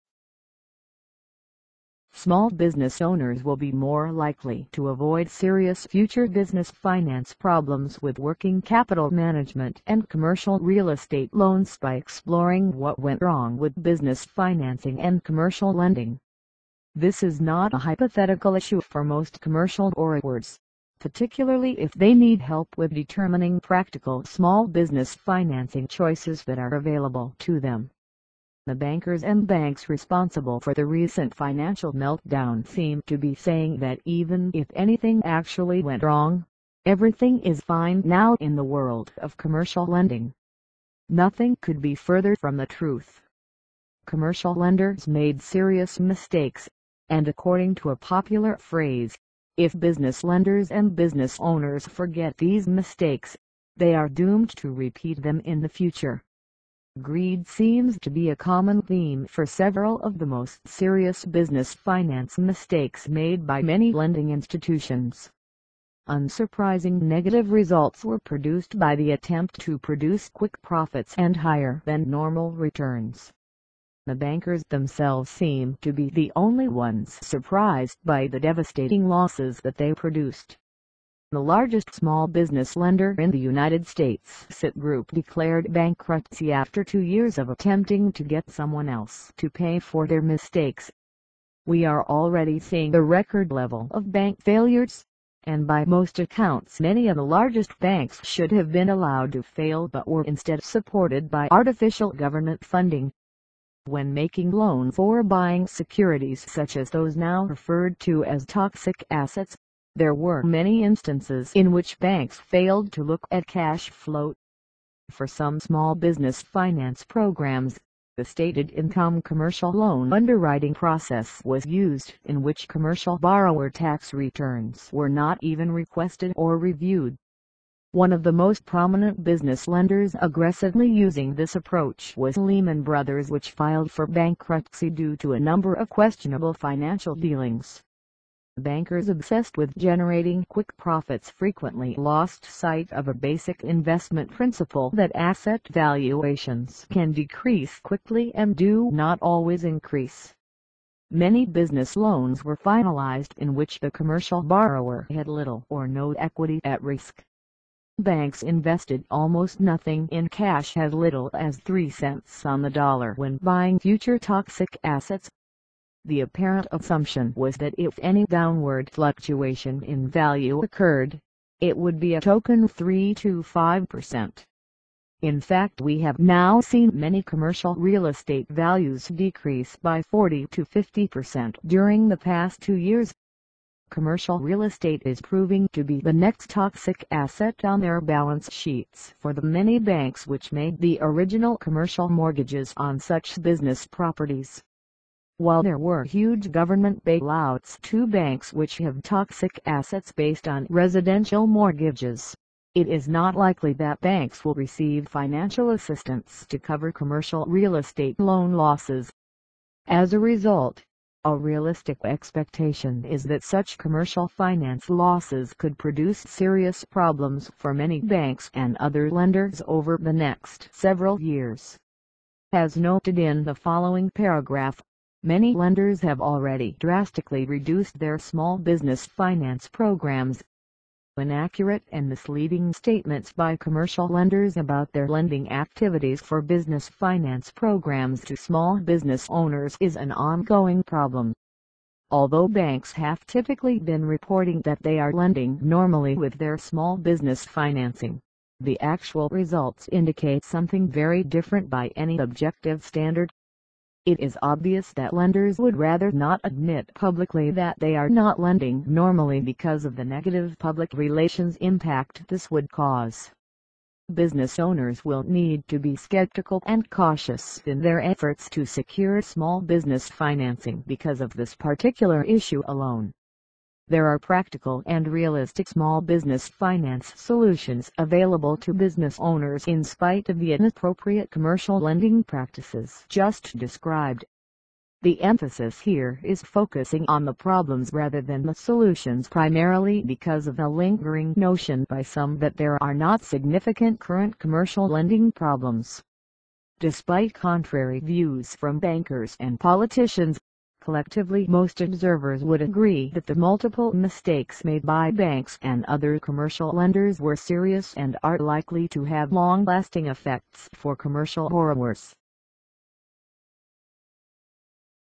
How Commercial Lenders Went Wrong With Small Business Financing
transcript

Small business owners will be more likely to avoid serious future business finance problems with working capital management and commercial real estate loans by exploring what went wrong with business financing and commercial lending. This is not a hypothetical issue for most commercial borrowers, particularly if they need help with determining practical small business financing choices that are available to them. The bankers and banks responsible for the recent financial meltdown seem to be saying that even if anything actually went wrong, everything is fine now in the world of commercial lending. Nothing could be further from the truth. Commercial lenders made serious mistakes, and according to a popular phrase, if business lenders and business owners forget these mistakes, they are doomed to repeat them in the future. Greed seems to be a common theme for several of the most serious business finance mistakes made by many lending institutions. Unsurprising negative results were produced by the attempt to produce quick profits and higher-than-normal returns. The bankers themselves seem to be the only ones surprised by the devastating losses that they produced. The largest small business lender in the United States (CIT Group) declared bankruptcy after two years of attempting to get someone else to pay for their mistakes. We are already seeing a record level of bank failures, and by most accounts many of the largest banks should have been allowed to fail but were instead supported by artificial government funding.

When making loans or buying securities such as those now referred to as toxic assets, there were many instances in which banks failed to look at cash flow. For some small business finance programs, a stated income commercial loan underwriting process was used in which commercial borrower tax returns were not even requested or reviewed. One of the most prominent business lenders aggressively using this approach was Lehman Brothers (which filed for bankruptcy due to a number of questionable financial dealings). Bankers obsessed with generating quick profits frequently lost sight of a basic investment principle that asset valuations can decrease quickly and do not always increase. Many business loans were finalized in which the commercial borrower had little or no equity at risk. Banks invested almost nothing in cash (as little as three cents on the dollar) when buying future toxic assets. The apparent assumption was that if any downward fluctuation in value occurred, it would be a token three to five percent. In fact we have now seen many commercial real estate values decrease by 40 to 50 percent during the past two years. Commercial real estate is proving to be the next toxic asset on their balance sheets for the many banks which made the original commercial mortgages on such business properties. While there were huge government bailouts to banks which have toxic assets based on residential mortgages, it is not likely that banks will receive financial assistance to cover commercial real estate loan losses. As a result, a realistic expectation is that such commercial finance losses could produce serious problems for many banks and other lenders over the next several years. As noted in the following paragraph, many lenders have already drastically reduced their small business finance programs. Although banks have typically been reporting that they are lending normally with their small business financing, the actual results indicate something very different by any objective standard. It is obvious that lenders would rather not admit publicly that they are not lending normally because of the negative public relations impact this would cause. Business owners will need to be skeptical and cautious in their efforts to secure small business financing because of this particular issue alone.

There are practical and realistic small business finance solutions available to business owners in spite of the inappropriate commercial lending practices just described. The emphasis here is focusing on the problems rather than the solutions primarily because of the lingering notion by some that there are not significant current commercial lending problems. Despite contrary views from bankers and politicians, collectively most observers would agree that the multiple mistakes made by banks and other commercial lenders were serious and are likely to have long-lasting effects for commercial borrowers.